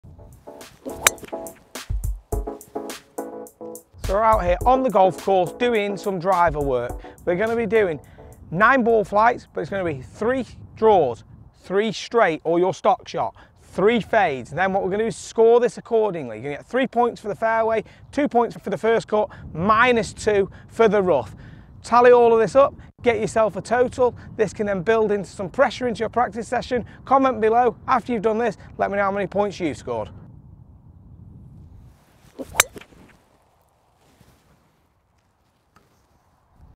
Test Your Driver Skills
Testing your golf game with skills tests is essential for your development. By practicing in this way you can put your game under pressure before you get out in a competition environment. This test involves hitting your driver at a fairway and should help you to gain accuracy on the golf course.